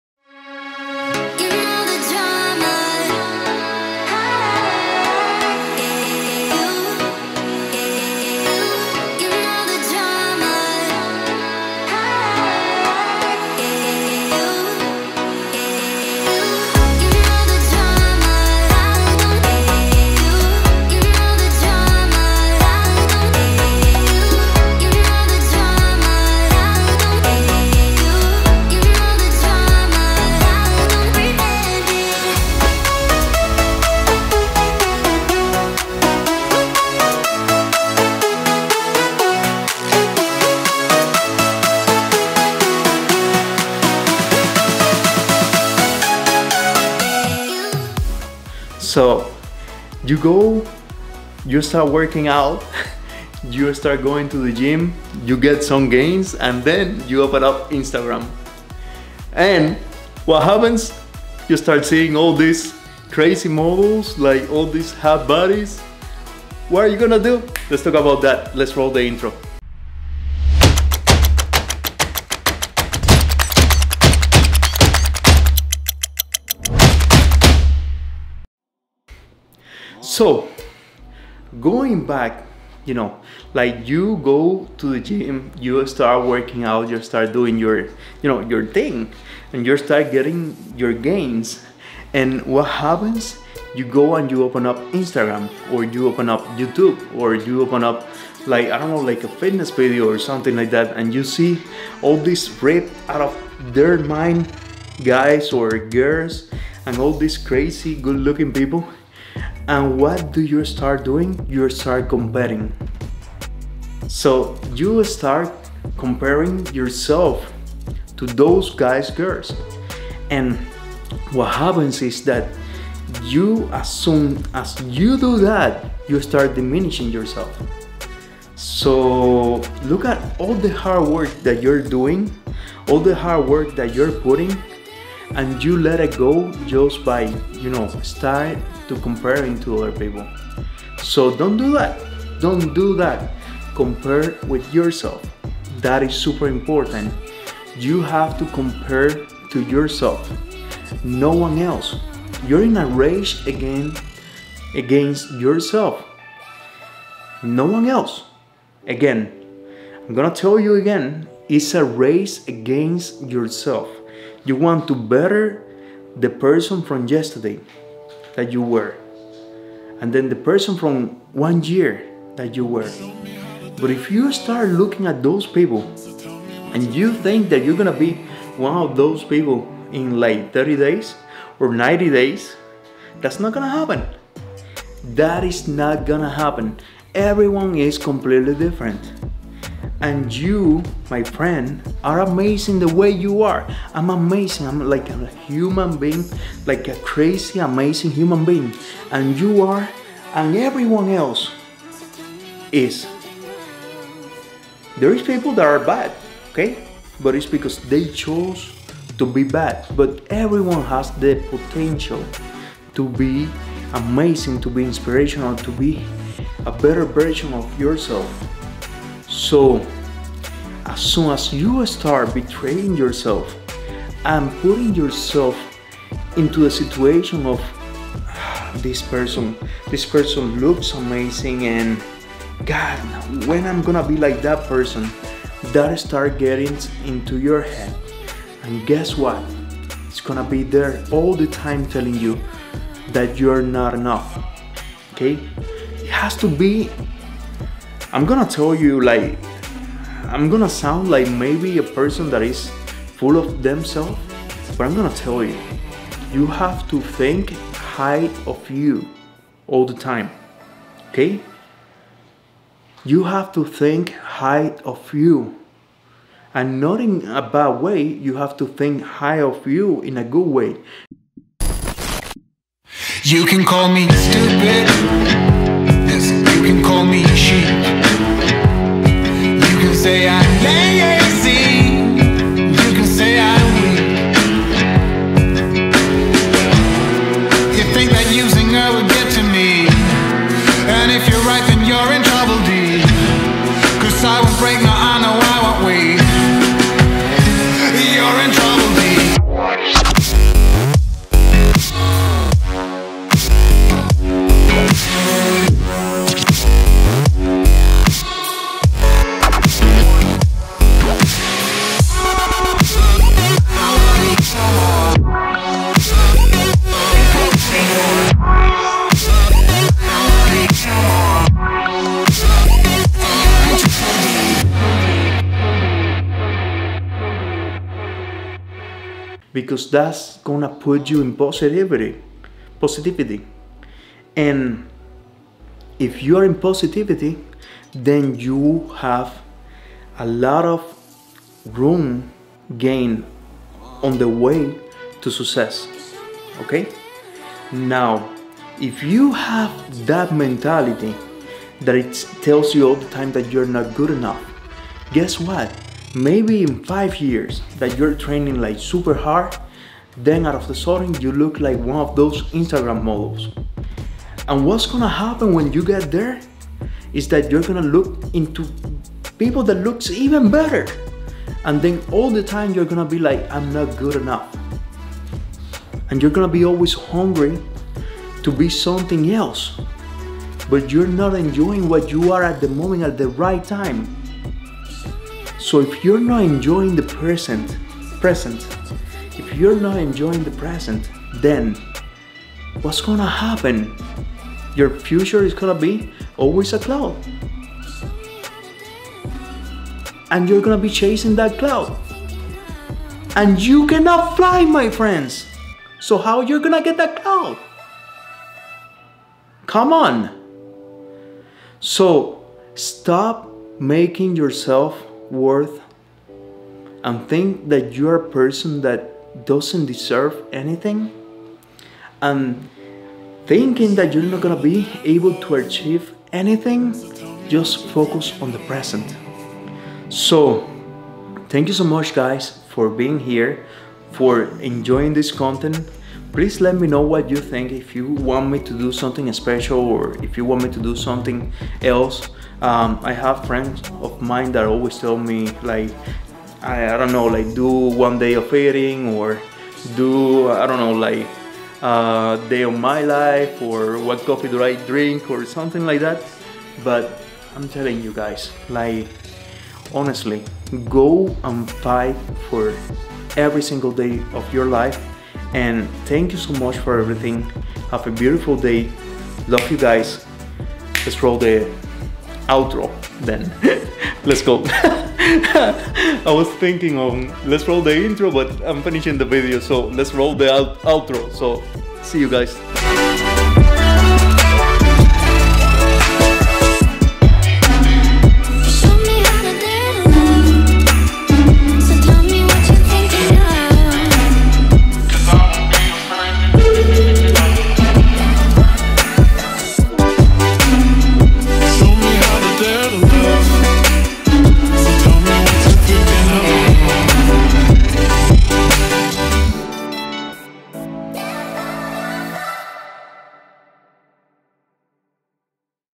YOU vs YOU Motivational | This is why you are not HAPPY?!
Hi! Today, I am going to share a video Vlog about You vs YOU. Watch this video completely to explorethe reason why you keep comparing to others instead of competing agains yourself. Keep watching and enjoying.

►Face book:
angelmenendezfitness